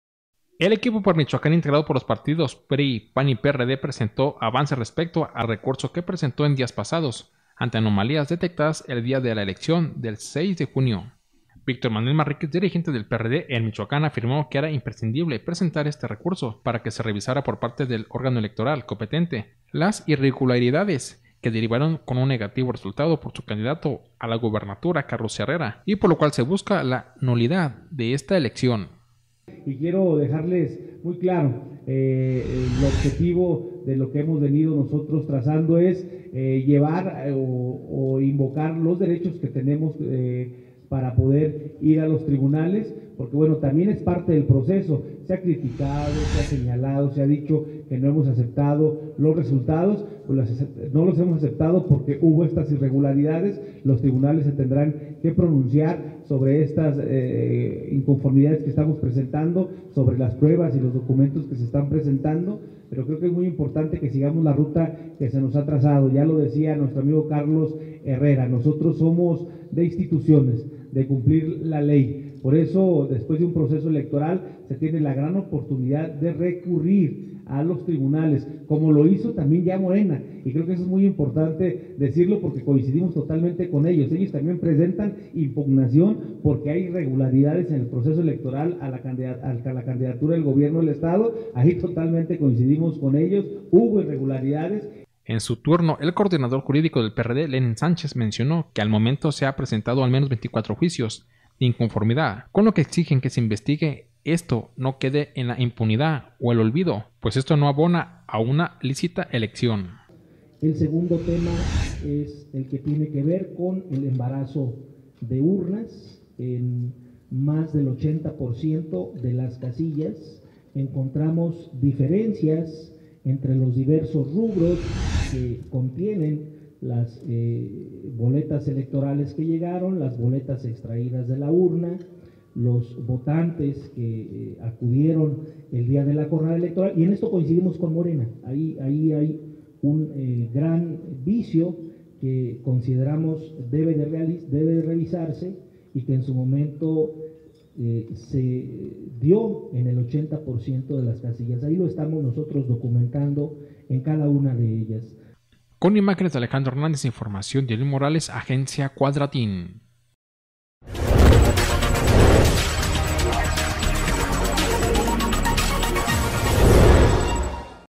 Suman 24 juicios de inconformidad para anular elección de Gobernador
MORELIA, Mich., 21 de junio de 2021.- El Equipo por Michoacán, integrado por los partidos Revolucionario Institucional (PRI), de la Revolución Democrática (PRD) y Acción Nacional (PAN) presentó avances respecto al recurso que se presentó en días pasados ante anomalías detectadas el día de la elección del pasado 6 de junio.